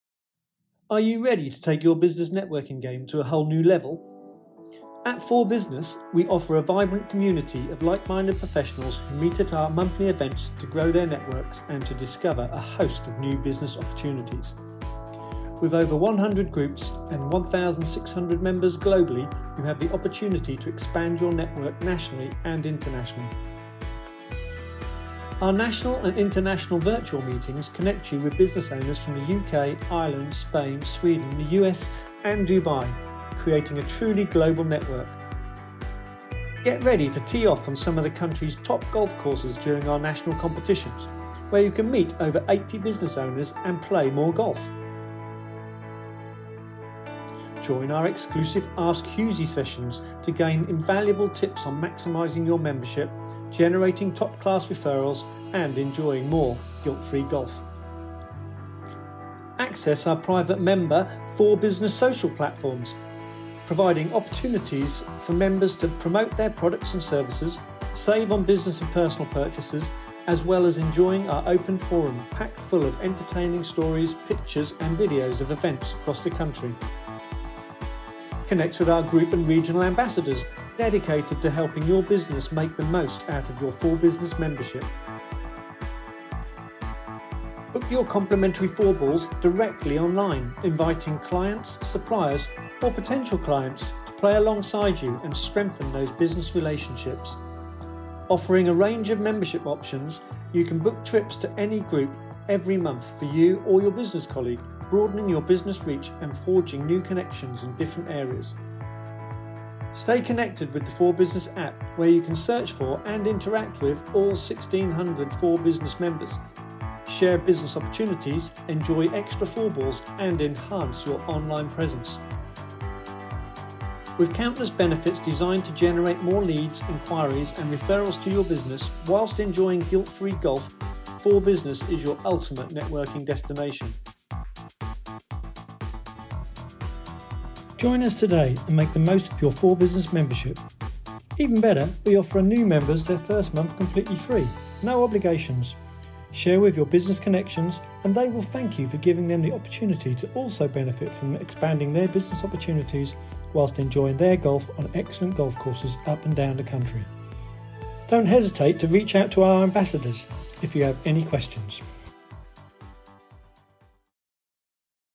FORE Business - Member Benefits
Are you ready to take your business networking efforts to a whole new level? FORE Business offers a vibrant community of like-minded professionals who convene at our monthly events to not only expand their networks but also uncover a plethora of new business opportunities.

With a global presence, boasting over 100 groups and 1600 members worldwide, you have the chance to broaden your network both nationally and internationally. Our national and international virtual meetings seamlessly connect you with fellow business owners hailing from the UK, Ireland, Spain, Sweden, the USA, and Dubai, thus creating a truly global network.

Prepare to tee off on some of the country's premier golf courses during our national competitions, providing the ideal setting to meet over 80 business owners while enjoying more rounds of golf.

Join our exclusive "Ask Hugh’sie" sessions, where you'll gain invaluable insights on how to maximize your membership, generate top-tier referrals, and indulge in guilt-free golf.

Access our private member-centric FORE Business social media platforms, providing opportunities for members to showcase their products and services, enjoy savings on business and personal purchases, and engage in our Open Forum filled with entertaining stories, pictures, and videos of events held across the country.

Forge connections with our dedicated Group and Regional Ambassadors, committed to helping your business get the most out of your FORE Business membership.

Book your complimentary four-balls directly online, allowing you to invite clients, suppliers, or potential clients to play alongside you and solidify those vital business relationships.

With a range of membership options, you can book trips to any group every month, either for yourself or a business colleague, broadening your business reach and establishing new connections in diverse regions.

Stay connected with the FORE Business APP, where you can search for and interact with all 1600+ FORE Business members, share business opportunities, enjoy extra four-balls, and bolster your online presence.

Offering a multitude of benefits designed to generate more leads, inquiries, and referrals for your business while relishing guilt-free golf, FORE Business stands as your ultimate networking destination.

Join us today and make the most of your FORE Business membership. What's even better? We extend the offer of the first month, completely free, to new members, with no obligations. Share this fantastic opportunity with your business connections - they'll thank you for giving them the chance to expand their business opportunities while relishing golf on excellent courses across the country. Should you have any questions, don't hesitate to reach out to our ambassadors.

FORE Business - Networking on the Fairway!